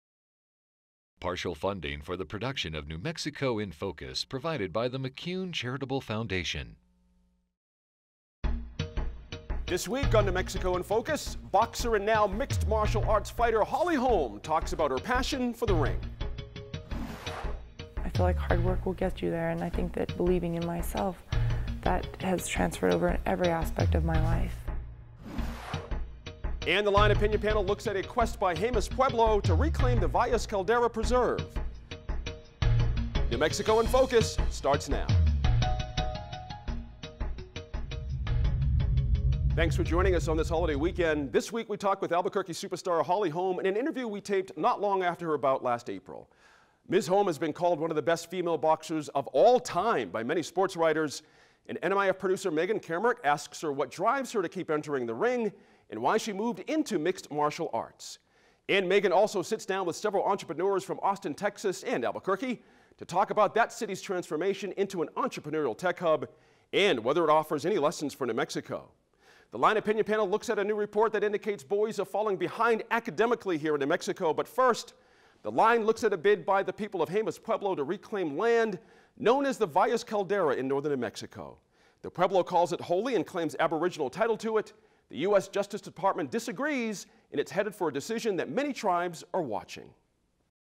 Episode 820 | Preview
This week Producer Megan Kamerick talks with undefeated fighter Holly Holm and we look at the rapid rise of Austin as a center of technology and entrepreneurship and what lessons it holds for Albuquerque and New Mexico. The Line panelists Dan Foley, Stephane Maez, Sophie Martin and Stephen Spitz discuss a claim to the Valles Caldera by Jemez Pueblo and a study indicating boys in New Mexico schools are lagging behind girls.